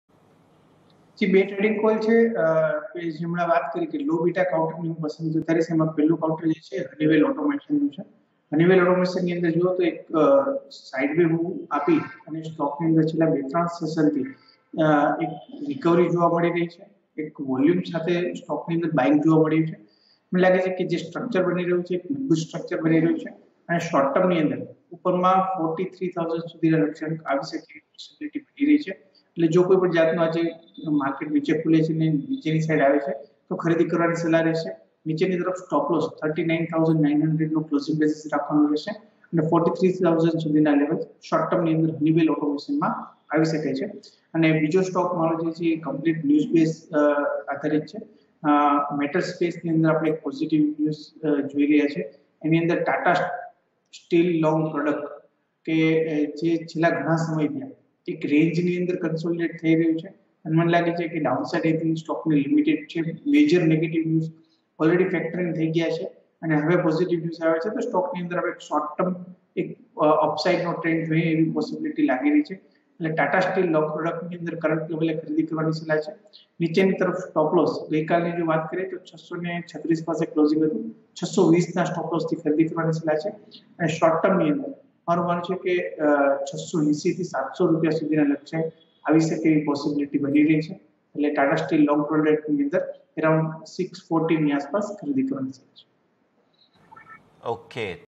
Buy Honeywell & Tata Steel Long Products | Trading call by Nimesh Thaker
CNBCBajar | Market માં આજે ક્યાં સ્ટોક કરાવશે કમાણી? જાણો nimeshthaker.com ના નિમેશ ઠાકર પાસેથી